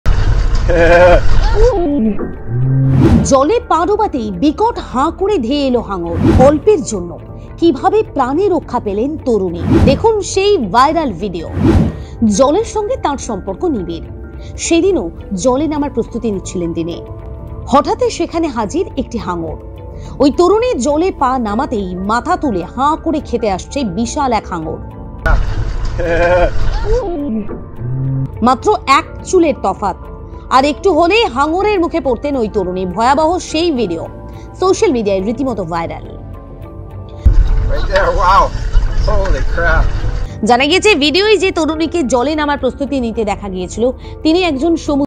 ধেয়ে এল হাঙর! Shocking video of marine scientist nearly diving into shark mouth
A video has gone viral where a diving expert can be seen to have narrowly avoided being chomped by a huge tiger shark as she was preparing to enter the waters in Hawaii. This moment was captured on camera. Ocean Ramsey is a marine researcher. She was about to dive into the waters near Oahu after she noticed that a shark bumped into a plastic pollution.

জলে পা ডোবাতেই বিকট হা করে ধেয়ে এল হাঙর! Shocking video of marine scientist nearly diving into shark mouth